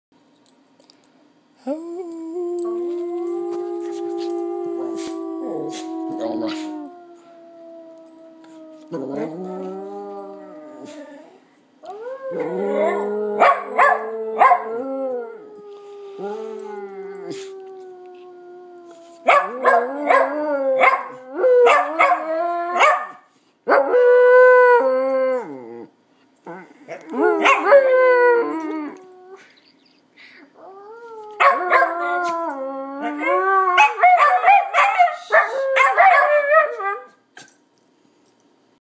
Westie dogs howl
Our two westies Goose and Gus, howl after a little provocation.